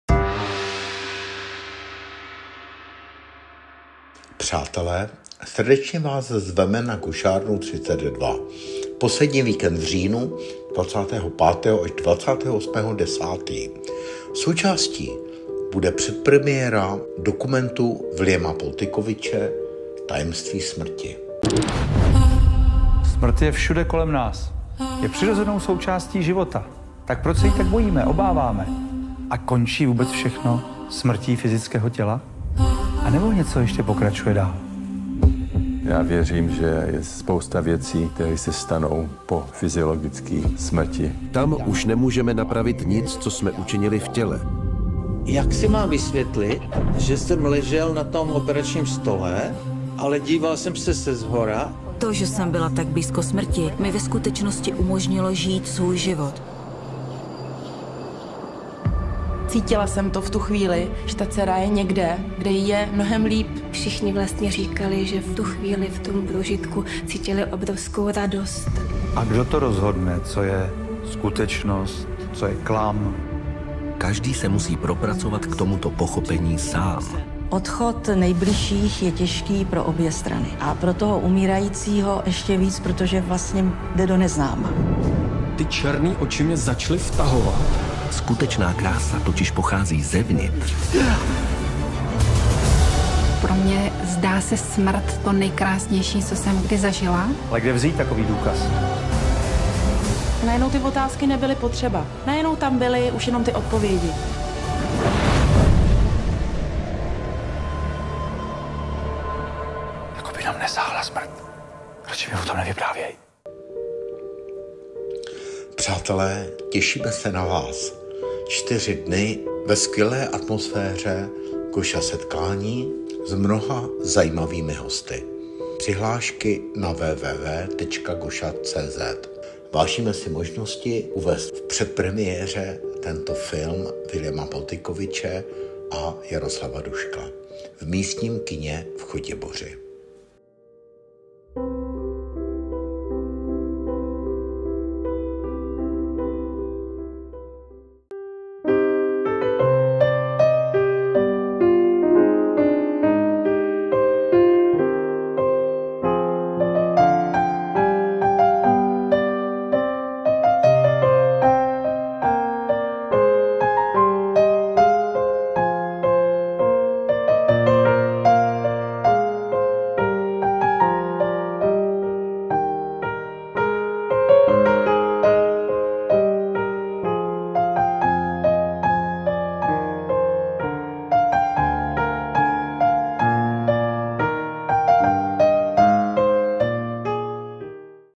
TAJEMSTVÍ SMRTI 💥 Viliama Poltikoviče a Jardy Duška🌟na Gošárně 32 - Srdečně ZVEME! 🌟25.-28. 10. 2024
NOVÝ dokument Viliama Poltikoviče na Gošárně 32. . Předpremiéra v kině v Chotěboři, v rámci programu spolku Goscha. - Přijeďte, těšíme se Vás. V čtyřdenním programu další zajímaví hosté na duchovní téma a hudba pro MEDITACI, radost i odlehčení: Patrik Kee a Standa Klapuch. 😊 NAŽIVO JE TO LEPŠÍ! 😊

Film TAJEMSTVÍ SMRTI v kinech od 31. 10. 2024